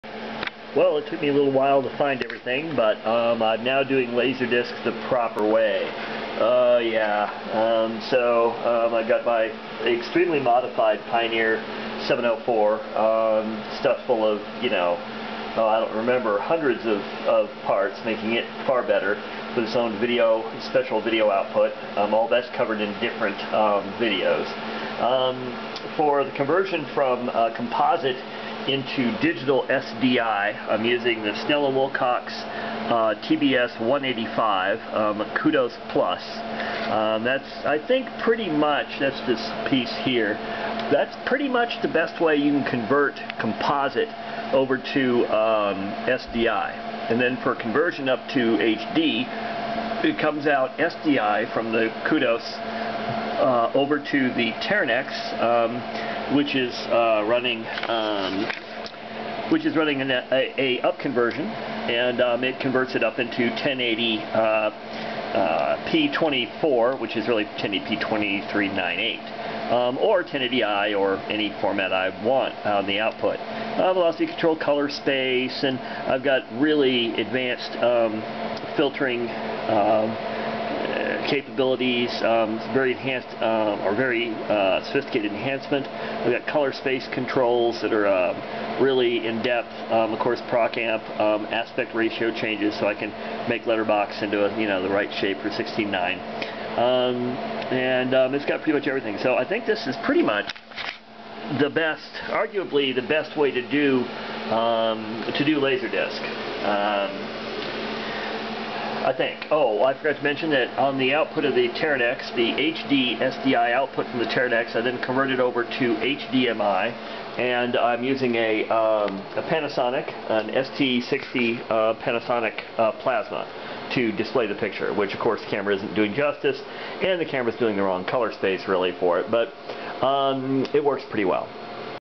Laser Disc Playback
My laser disc playback chain. A Snell & Wilcox TBS185 feeding a Teranex that feeds my Panasonic plasma.